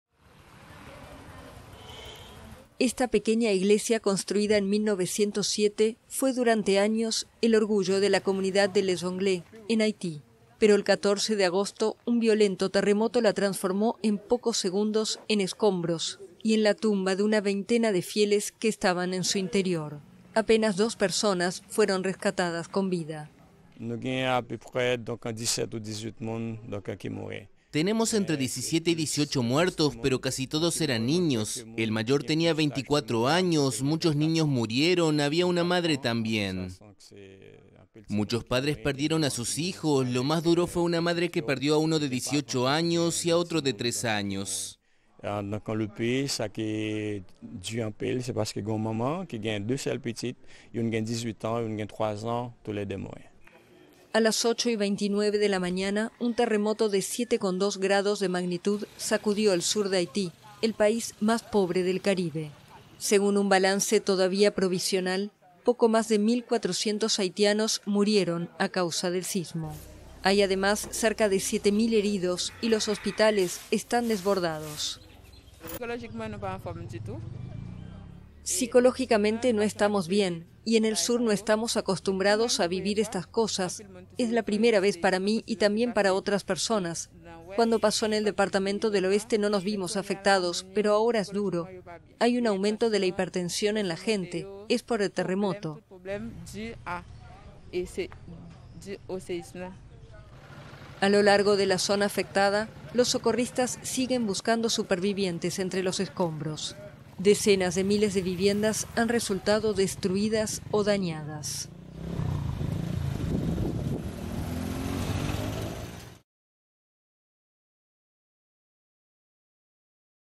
Destruida por el sismo, una iglesia de Haití llora sus muertos | AFP
Con su campanario y sus muros amarillos, la iglesia de la Inmaculada Concepción resplandeció durante años en la comunidad de Les Anglais, en Haití. Pero en pocos segundos, el 14 de agosto un violento terremoto la transformó en una tumba para cerca de una veintena de fieles que estaban en su interior.